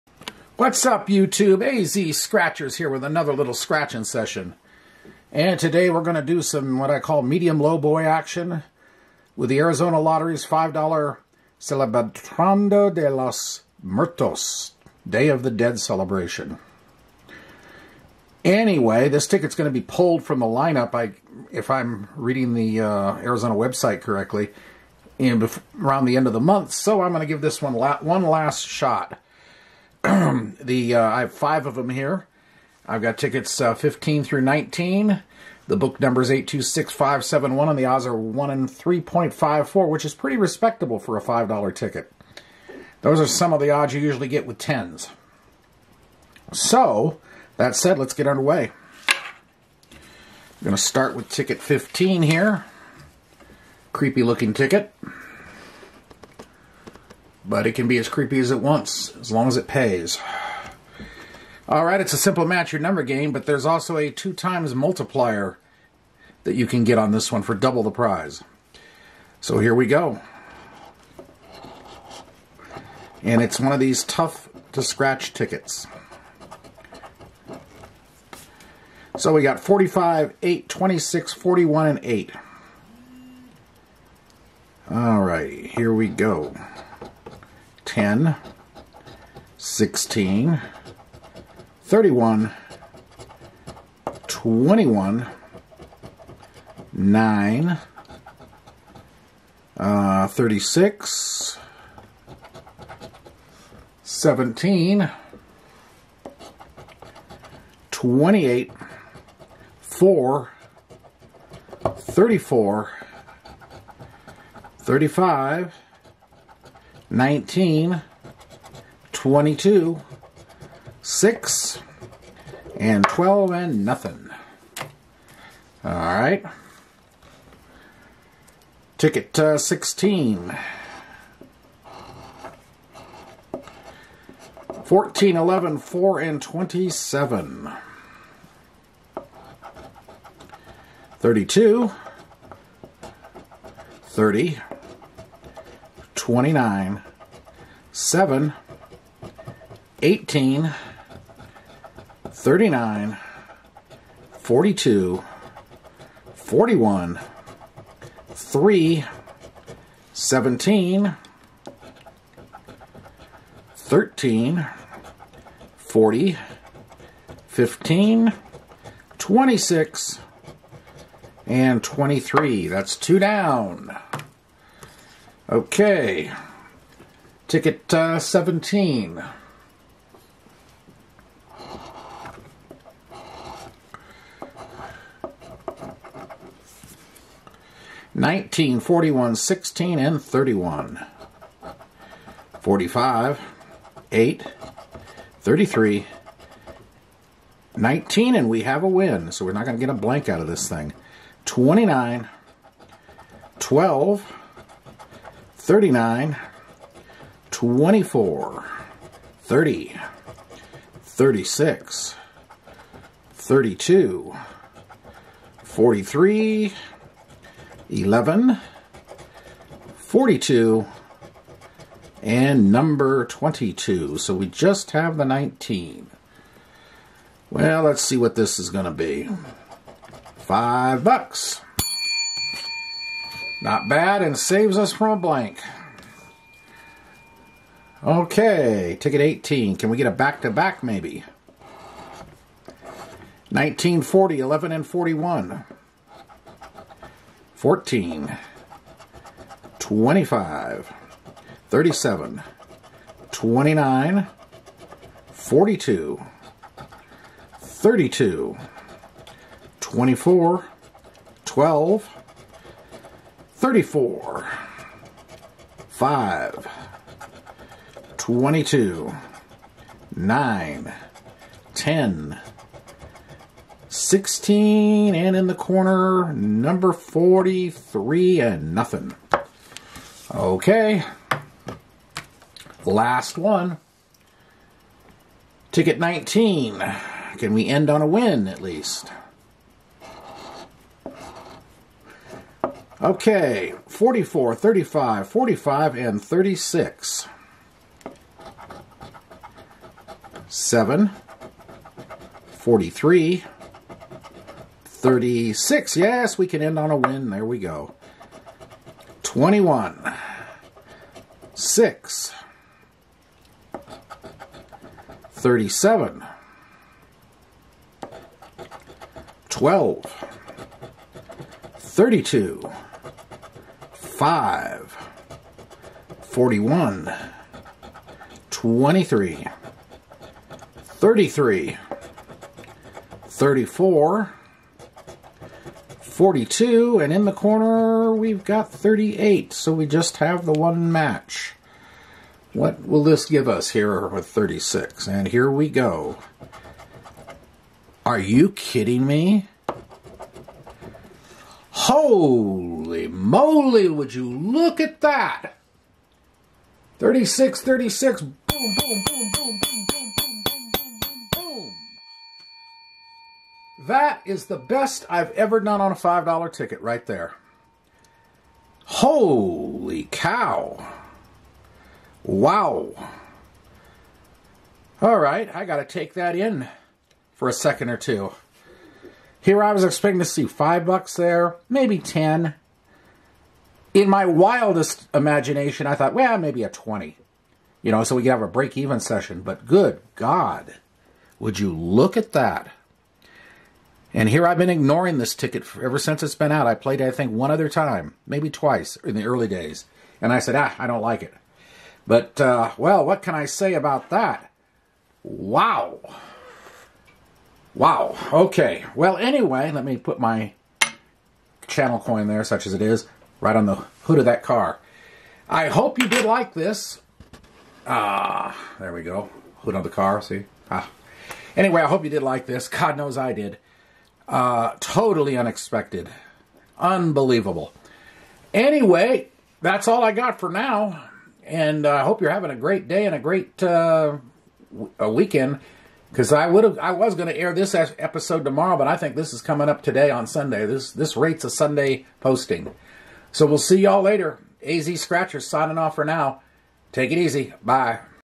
Five $5 Scratchers. 💥💥MASSIVE PROFIT SESSION💥💥😂
Biggest win ever on a $5 ticket!!! 1 in 6,013,95 odds!! 🎯😎